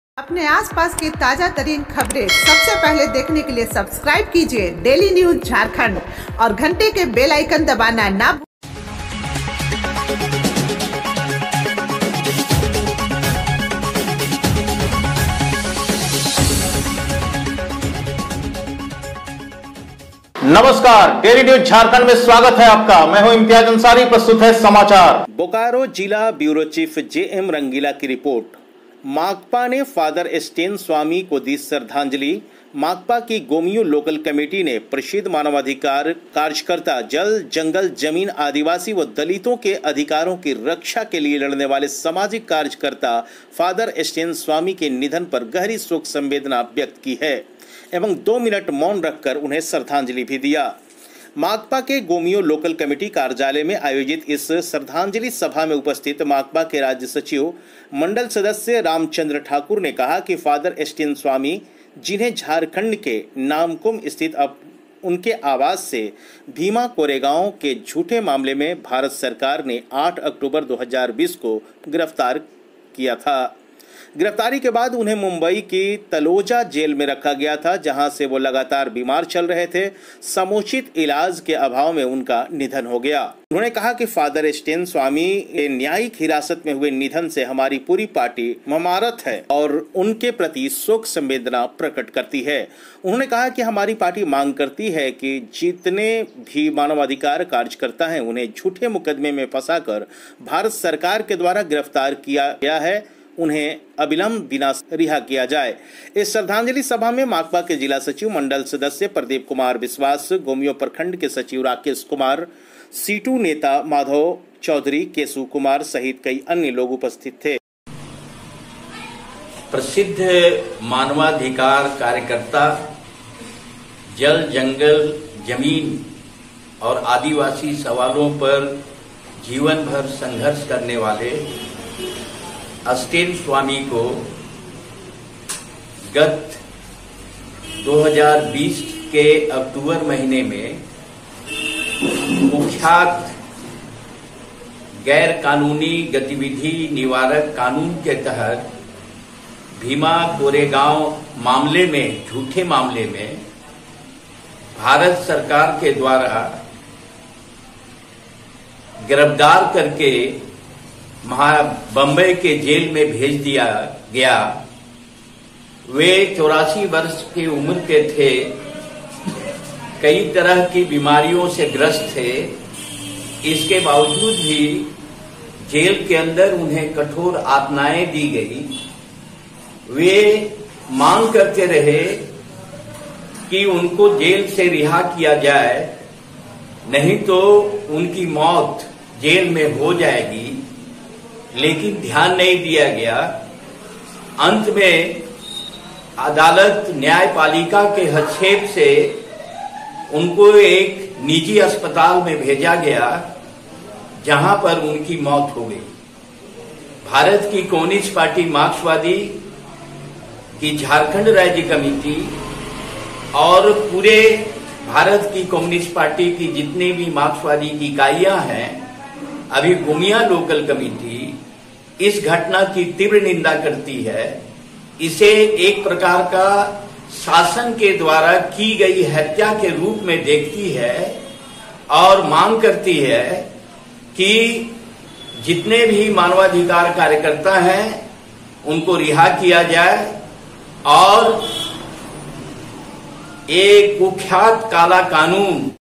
माकपा ने फादर स्टेन स्वामी को दी श्रद्धांजलि। - DAILY NEWS JHARKHAND
माकपा ने फादर स्टेन स्वामी को दी श्रद्धांजलि।
माकपा की गोमिया लोकल कमेटी ने प्रसिद्ध मानवाधिकार कार्यकर्ता,जल-जंगल-जमीन, आदिवासी व दलितों के अधिकारों की रक्षा के लिए लड़ने वाले सामाजिक कार्यकर्ता फादर स्टेन स्वामी के निधन पर गहरी शोक संवेदना व्यक्त की एवं दो मिनट मौन रखकर उन्हें दी श्रद्धांजलि।
       माकपा के गोमिया लोकल कमेटी कार्यालय में आयोजित इस श्रद्धांजलि सभा में उपस्थित माकपा के राज्य सचिव मंडल सदस्य रामचंद्र ठाकुर ने कहा की फादर स्टेन स्वामी जिन्हें झारखंड के नामकुम स्थित उनके आवास से भीमा कोरेगांव के झूठे मामले में भारत सरकार ने 8 अक्टूबर 2020 को गिरफ्तार किया गया था। गिरफ्तारी के बाद उन्हें मुंबई की तलोजा जेल में रखा गया था, जहां से वह लगातार बीमार चल रहे थे।  समुचित इलाज के अभाव में उनका निधन हुआ। उन्होंने कहा फादर स्टेन स्वामी के न्यायिक हिरासत में हुए निधन से हमारी पूरी पार्टी मर्महत है और उनके प्रति शोक संवेदना प्रकट करती है।
         उन्होंने कहा कि हमारी पार्टी मांग करती है कि जितने भी मानवाधिकार कार्यकर्ता हैं जिन्हें झूठे मुकदमे में फंसाकर भारत सरकार के द्वारा गिरफ्तार किया गया है उन्हें अविलंब बिना शर्त रिहा किया जाए।
        इस श्रद्धांजलि सभा में माकपा के जिला सचिव मंडल सदस्य प्रदीप कुमार विश्वास, गोमिया प्रखंड के सचिव राकेश कुमार, सीटू नेता माधव चौधरी, केशु कमार सहित कई लोग उपस्थित थे।